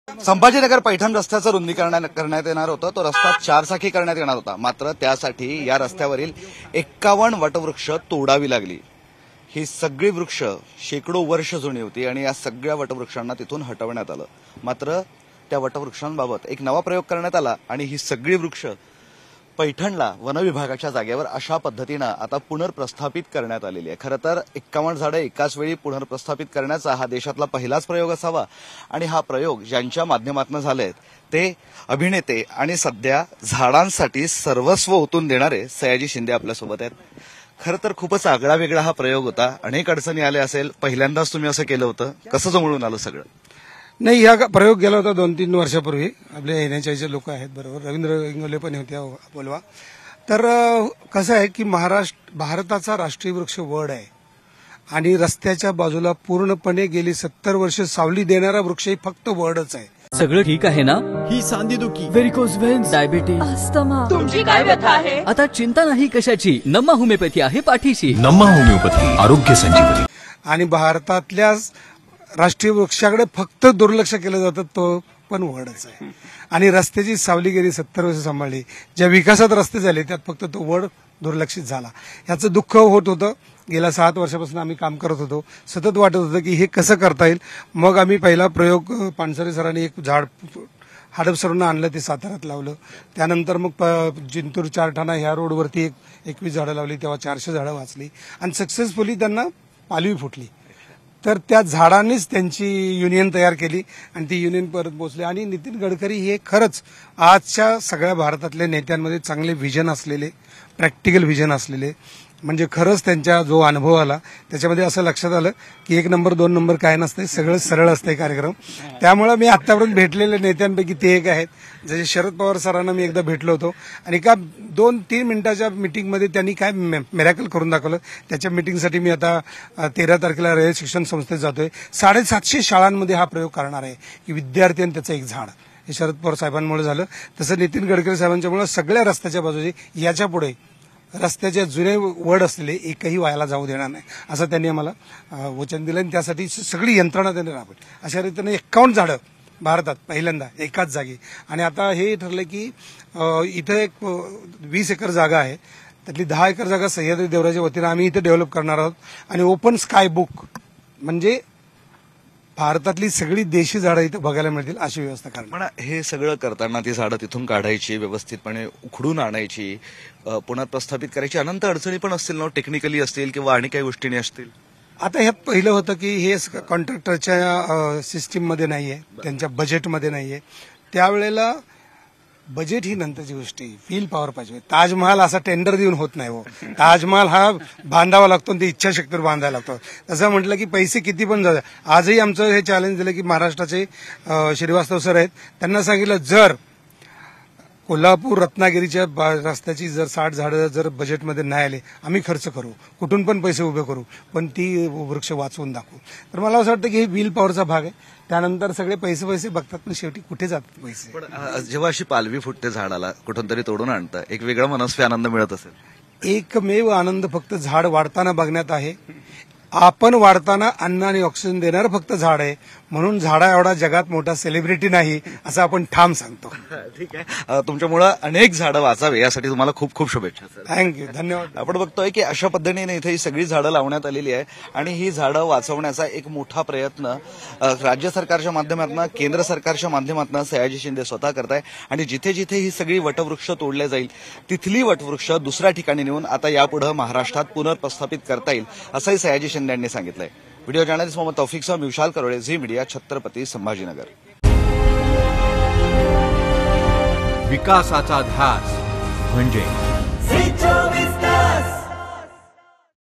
Sambhajinagar | 51 वटवृक्ष पुर्नप्रस्थापित करण्याचा प्रयोग, Sayaji Shinde यांच्या प्रयत्नांना यश
51 Banyan tree restoration experiment, Sayaji Shinde's efforts succeed

Social Media Links :
5 Minitat 25 Batmya । 5 मिनिटांत 25 बातम्या